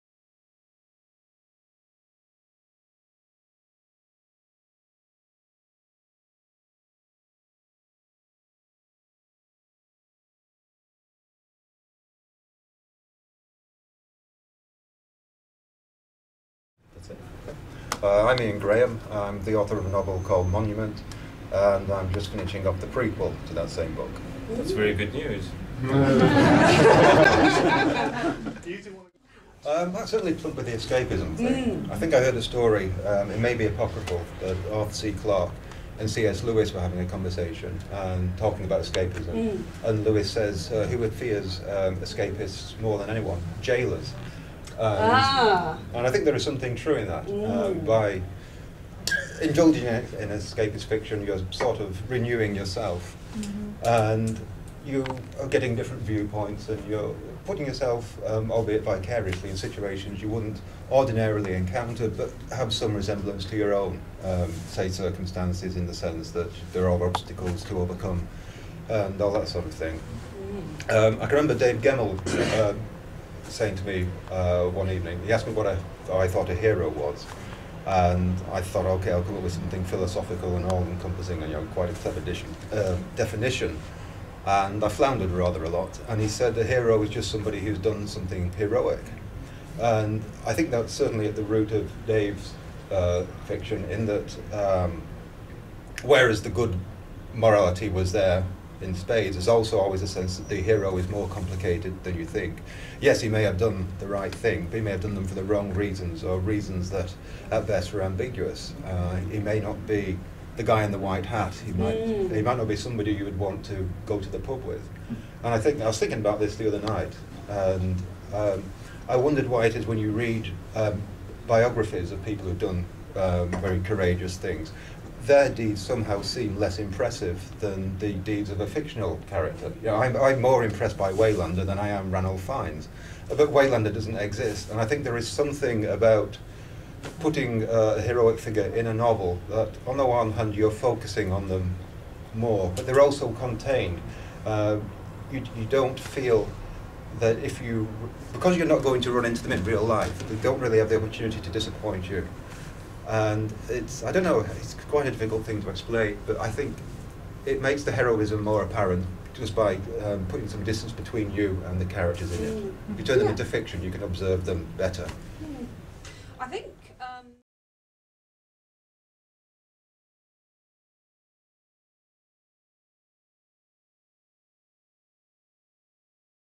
IanGraham Fantasycon2009 - on a panel about David Gemmell and Heroic Fantasy
Ian Graham waxes lyrical about David Gemmell and Heroic Fantasy.